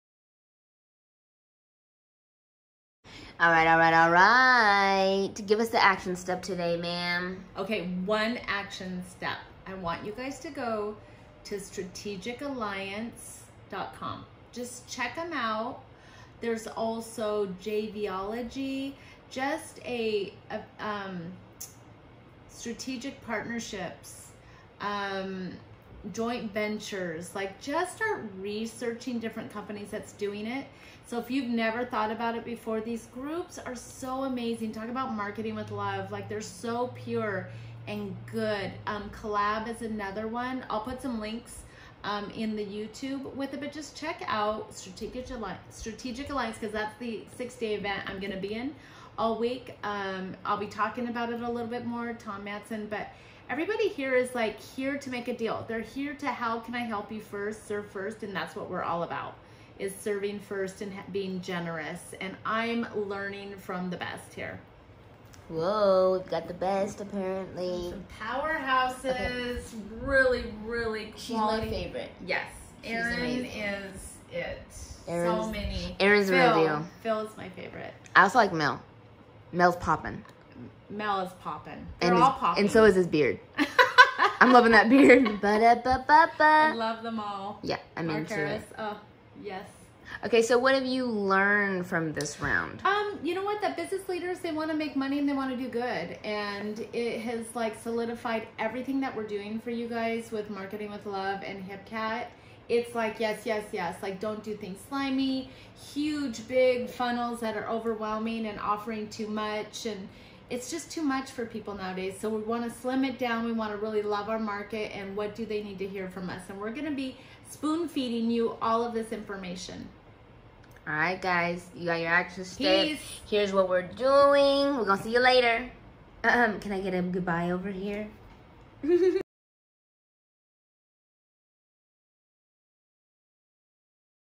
Day 6: Strategic Alliance
Your action step for today:
Define and Research what a good strategic alliance looks like for you.

We:
Are part of and will be sharing how we're gonna build on the momentum in the follow-up.

Be sure to sign up for the HipCat app, and sample it for FREE!

HIPCAT SOCIETY
1721 Hewitt Ave., Suite 501